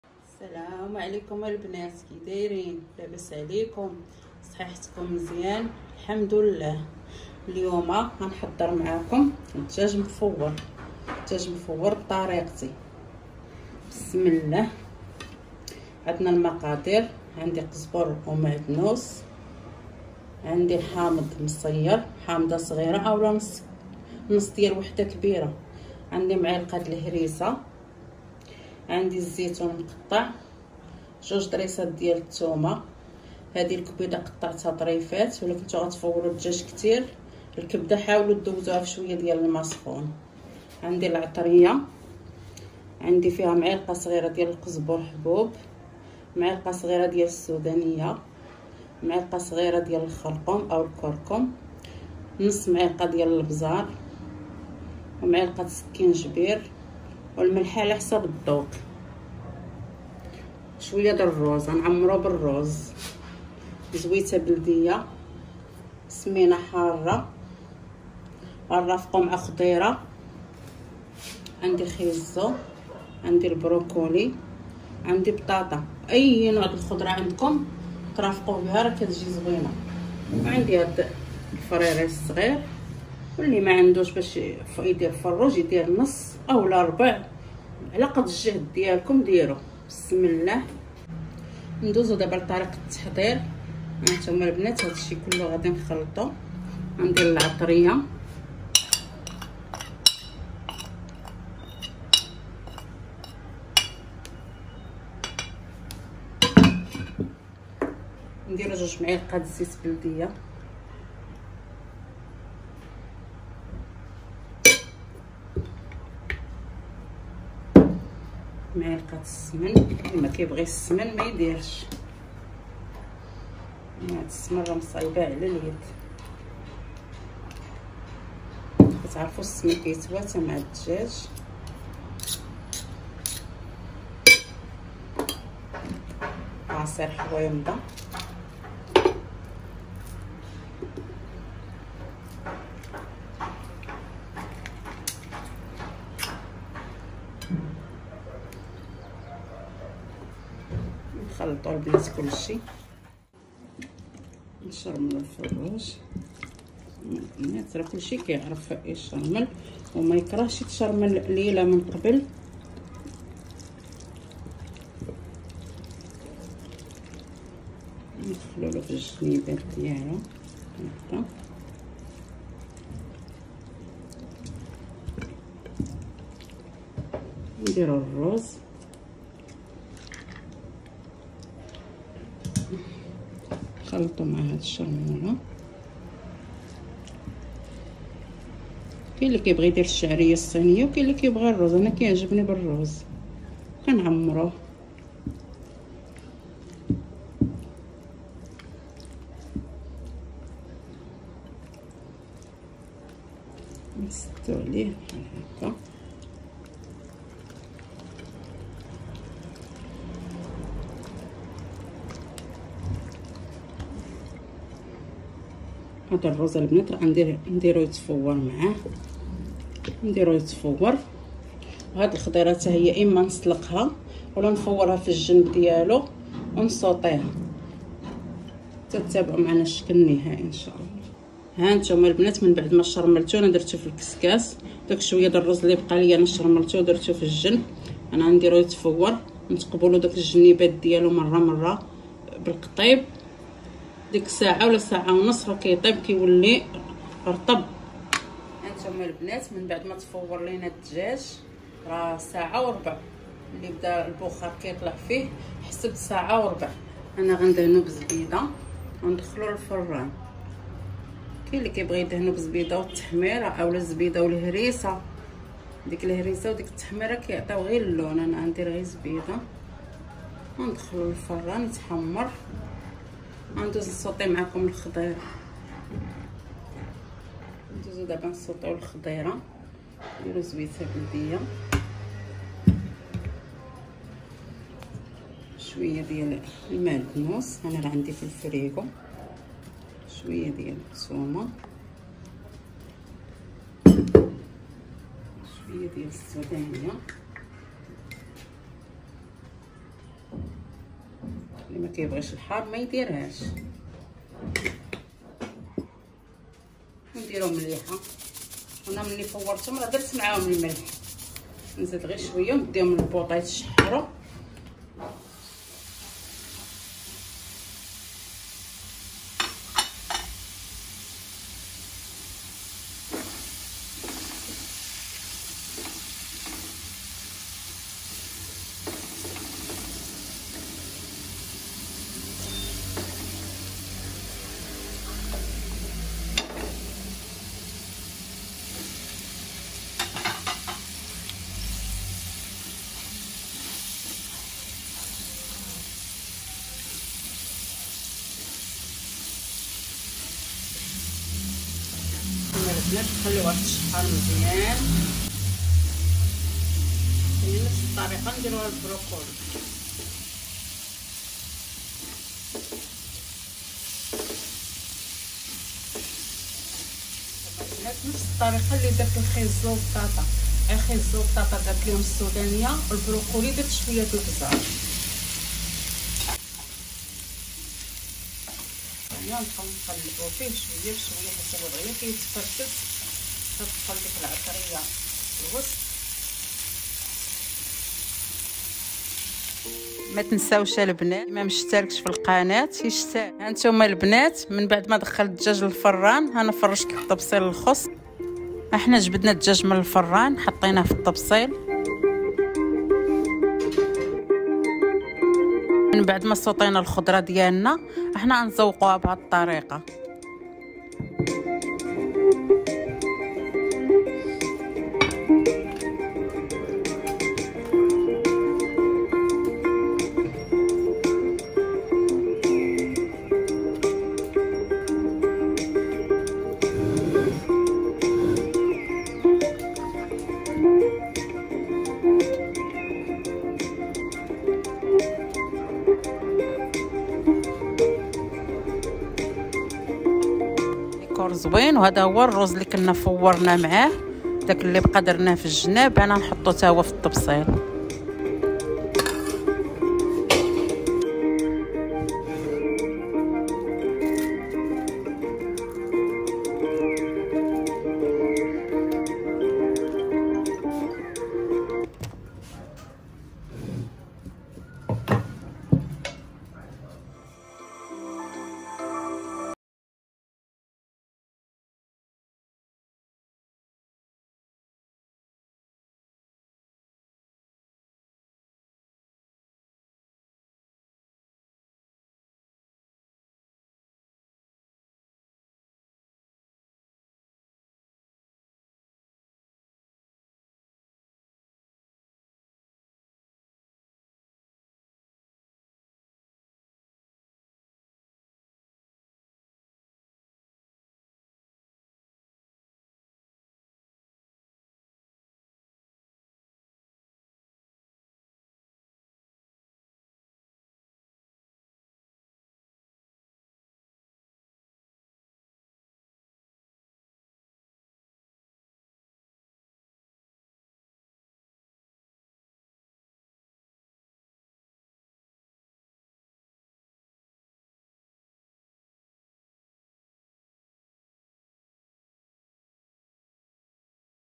دجاج مفور و محمر بطريق رائعة عمرك شفتيها ، طريقة كتحمق الضياف لي كيجيو عندي كتحمقهم
قناة يوميات بشرى قناة للطبخ المغربي التقليدي الاصيل